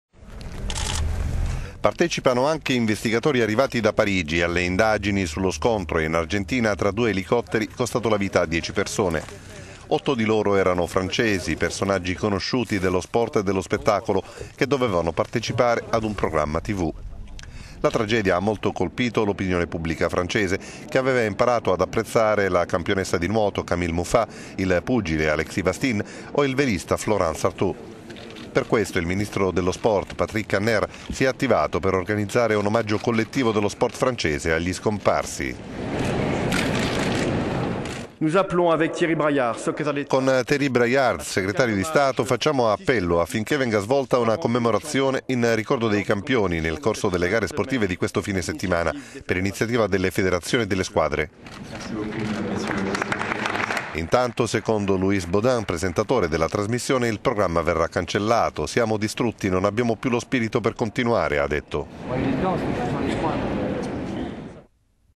La Francia sportiva ricorderà gli atleti morti nello scontro di elicotteri in Argentina
Partecipano anche investigatori arrivati da Parigi, alle indagini sullo scontro, in Argentina, tra due elicotteri costato la vita a dieci persone.

Otto di loro erano francesi, personaggi conosciuti dello sport e dello spettacolo che dovevano partecipare a un programma tv.

La tragedia ha molto colpito l'opinione pubblica francese, che aveva imparato ad apprezzare la campione di nuoto Camille Muffat, il pugile Alexis Vastine, o il velista Florence Artoud.